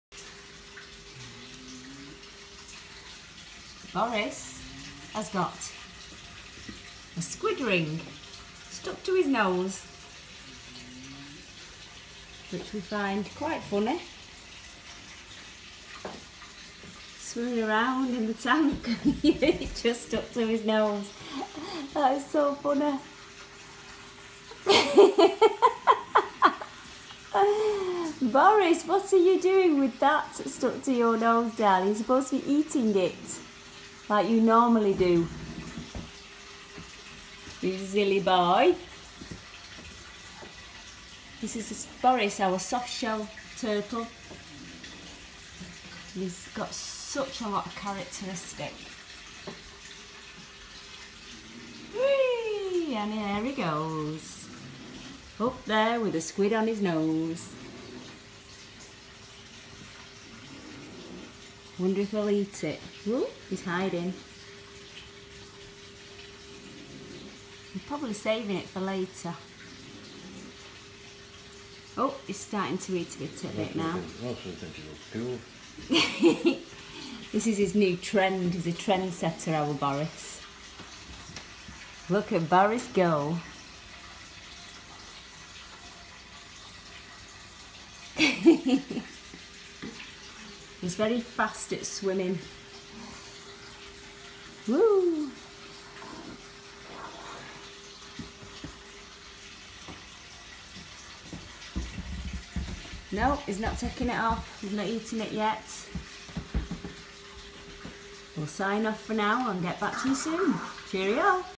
Boris’s nose ring.
Boris the soft shell turtle with a squid ring stuck on his nose.